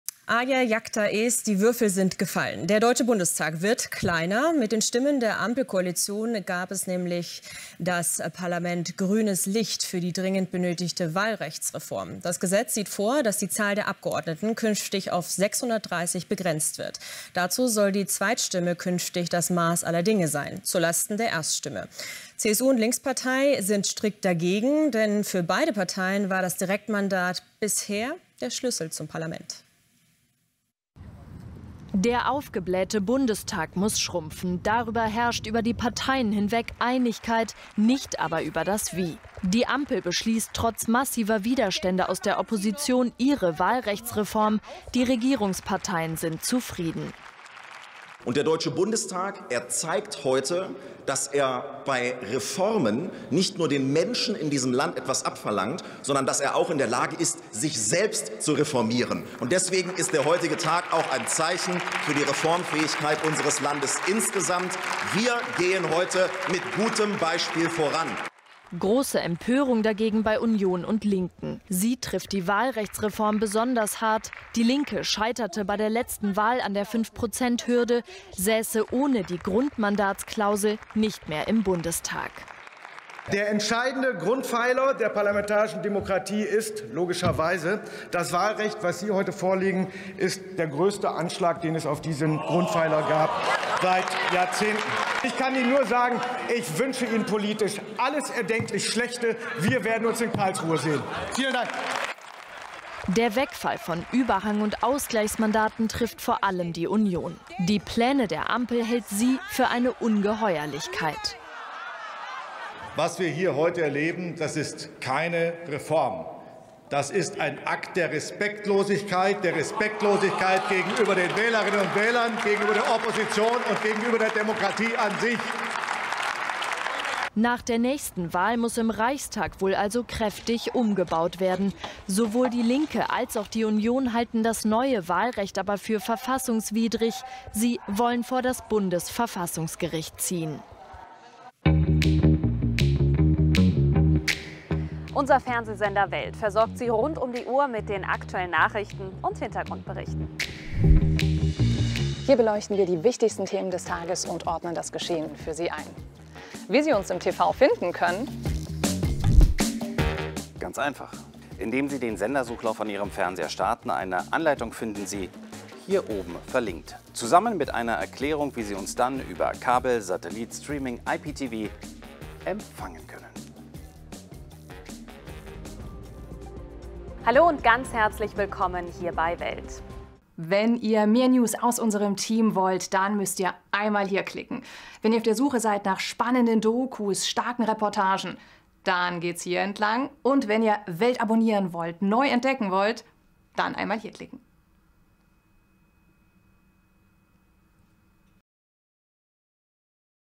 PARLAMENT SOLL SCHRUMPFEN: Bundestag beschließt Wahlrechtsreform - Linke und Union stinksauer
PARLAMENT SOLL SCHRUMPFEN: Bundestag beschließt Wahlrechtsreform-Linke und Union bangen

Die große Frage bei dieser Reform des Wahlrechts ist: Wie viele Abgeordnete sollen dem deutschen Volke dienen ? An sich ja knapp sechshundert, aber weit über siebenhundert sind es jetzt, im größten Bundestag aller Zeiten mit genau 736 Abgeordneten.
Das ist eigentlich allen zu viel, zumal es auch noch mehr werden könnten. Über die Jahre entstand aber ein kompliziertes System, das keinen benachteiligen soll.
Die jetzige Änderung soll den Bundestag wieder um gut hundert Sitze kleiner machen. Die Regierungsparteien sind zufrieden.

Dieses Ausgleichssystem kommt weg, aber dann kommt vielleicht nicht mehr jeder Wahlkreissieger auch in den Bundestag: schlecht vor allem für die CSU mit vielen Wahlkreissiegern, aber bundesweit wenigen Stimmen.

Was sich ebenfalls noch ändert: Die Fünfprozenthürde gilt absolut. Bisher musste eine Partei vor allem drei Wahlkreise direkt gewinnen, dann waren die fünf Prozent egal, sie kam doch in voller Stärke in den Bundestag, eine Spezialität der Linken
Auch damit soll aber Schluss sein, für Linke und Union sind die Vorschläge eine Ungeheuerlichkeit.
Nach der nächsten Wahl müsste im Reichstag also wieder kräftig umgebaut werden. Sowohl die Linke als auch die Union wollen aber gegen das neue Wahlrecht vor das Bundesverfassungsgericht ziehen.